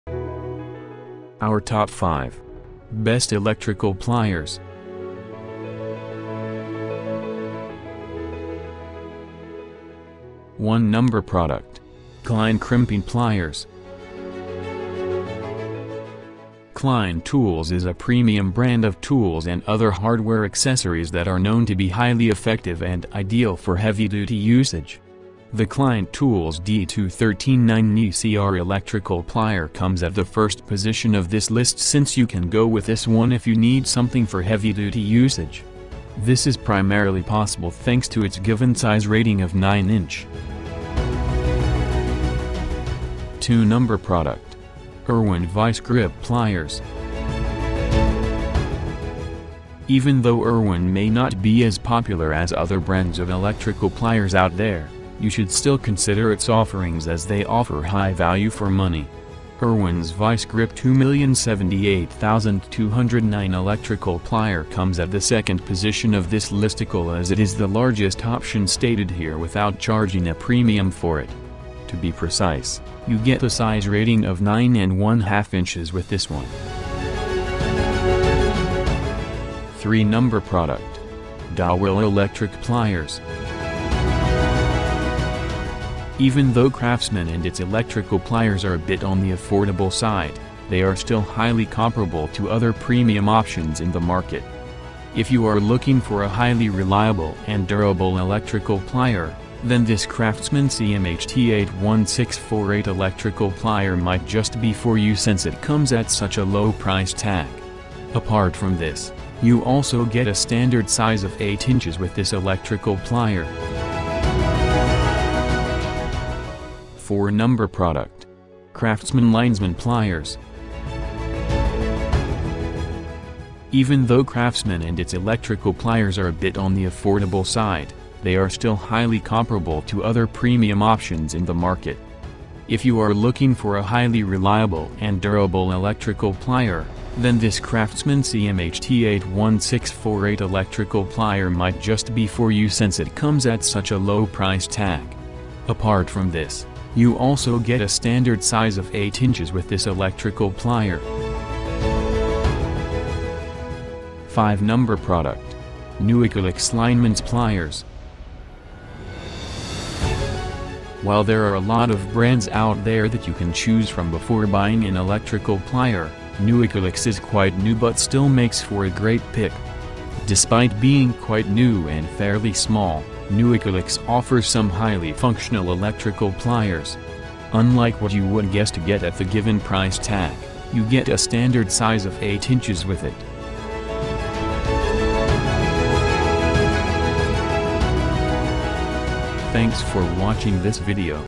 ✅Best Electrical Pliers || [Top 5 Pikes For You]
Do you need the Best Electrical Pliers for PS5 for You in the market? Don’t worry. Because in this guide, we will discuss everything you need to know about purchasing the Best Electrical Pliers in this review. I’m going to show you the five Electrical Pliers out there. Ultimately, you’re going to discover which Electrical Plier is best for you. Here we have listed the Best Electrical Pliers in the market, just see and pick one from our list as needed. So, Let’s go. We hope this review will be very helpful for you. Thank you.

✅Best Electrical Pliers list is here:

1. Klein Tools D213-9NE-CR Lineman's Crimping Pliers

2. IRWIN VISE-GRIP Lineman's Pliers

3. DOWELL Lineman's Pliers Combination Pliers

4. CRAFTSMAN Linesman Pliers

5. Lineman's Pliers, Big Combination Pliers